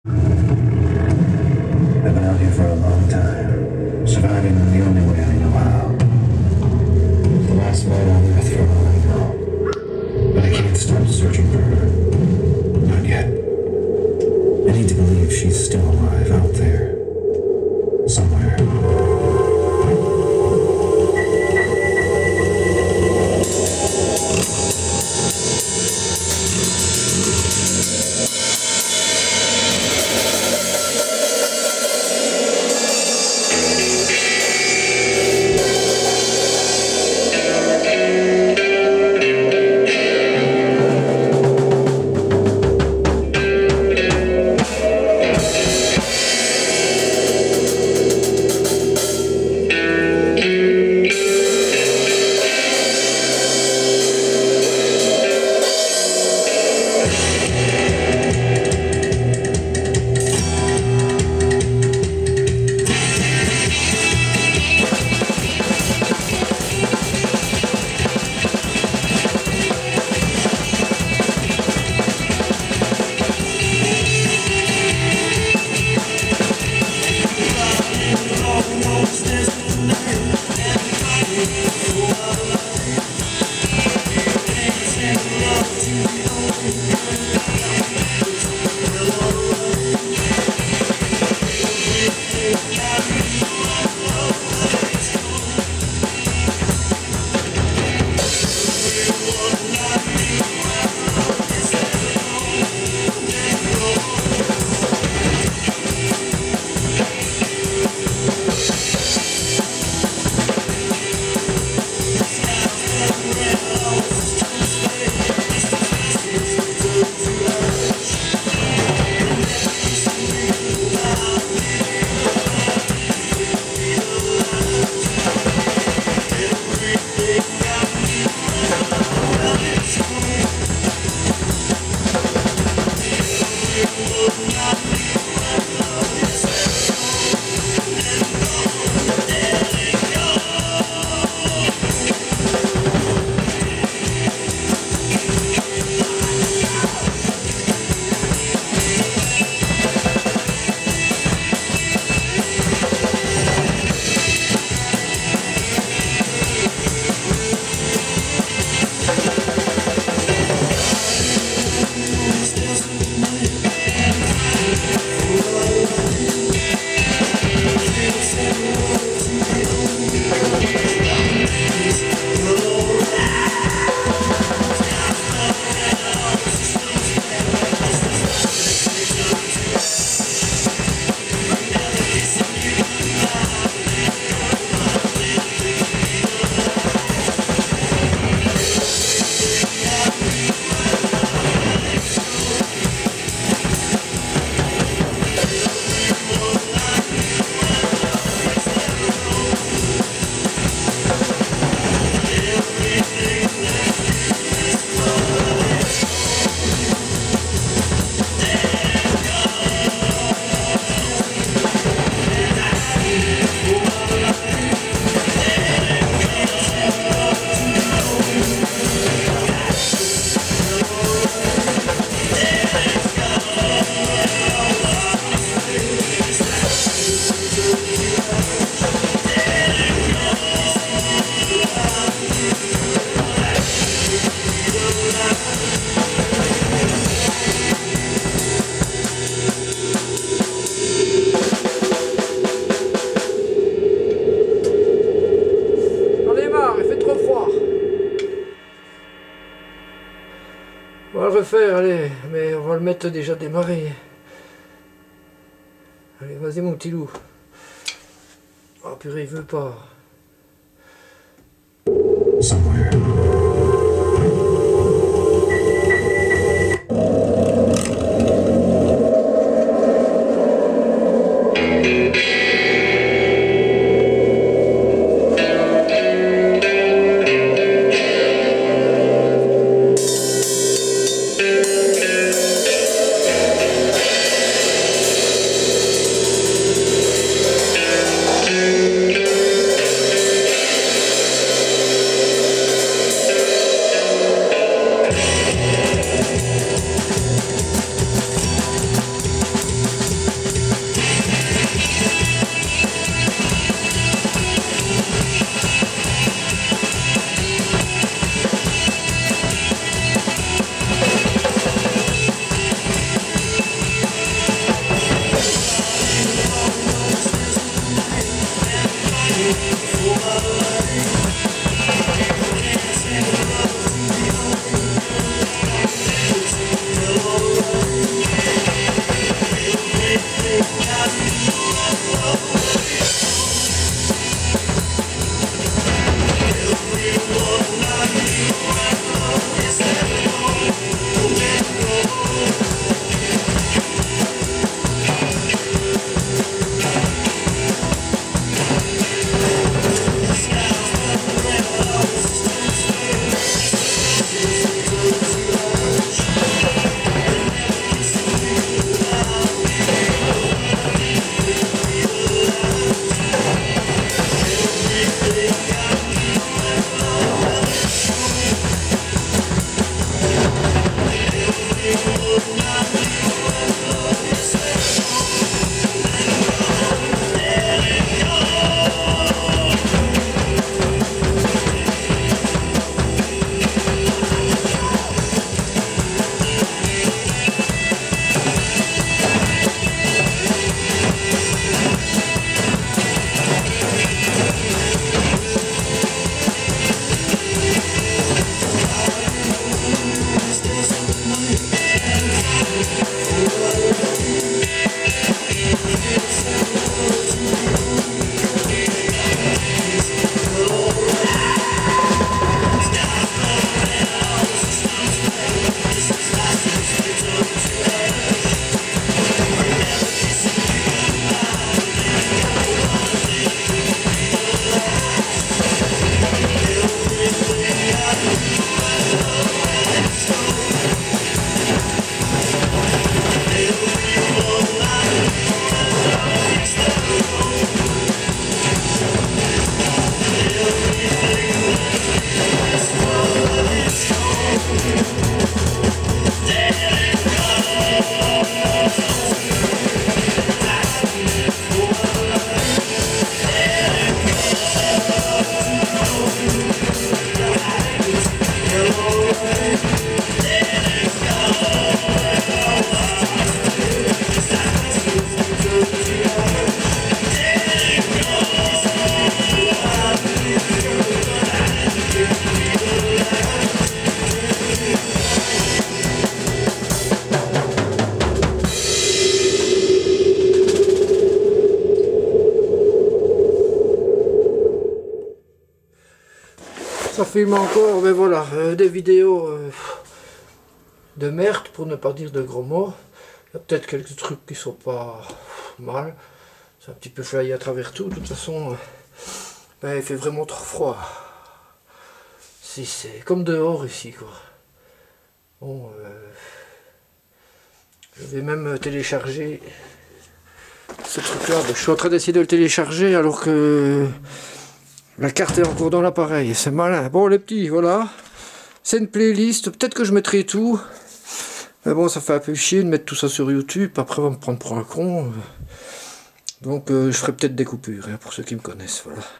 rock joué 2 fois ( raté ! ) 10 vidéos faites un vendredi par moins 10 degrés !
bonjour voila un essais ,mettre tout ce que j'ai joué un après midi,  vite fait !! c'est pas l'idéal mais je voulais voir ! prochaine surement un morceaux a la fois et les trop ratés ! poubelles mdrrrrr